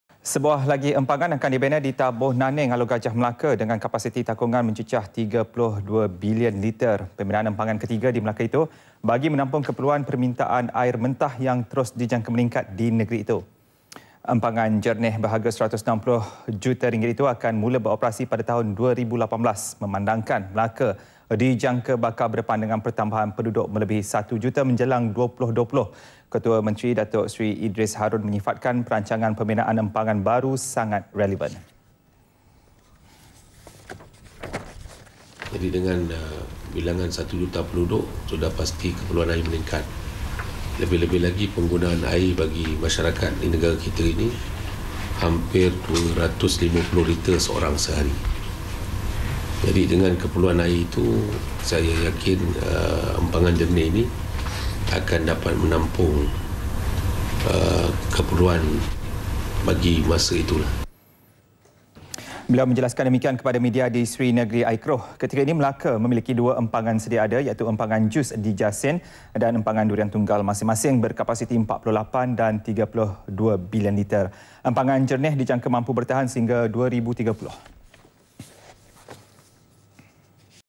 MELAKA CADANG BINA EMPANGAN : DATUK SERI IDRIS HARON [15 Jan 2015]
Sebuah lagi empangan akan dibina di Taboh Naning, Alor Gajah, Melaka dengan kapasiti takungan mencecah 32 billion liter.

Pembinaan empangan ke-3 di Melaka itu bagi menampung keperluan permintaan air mentah yang terus dijangka meningkat di negeri itu.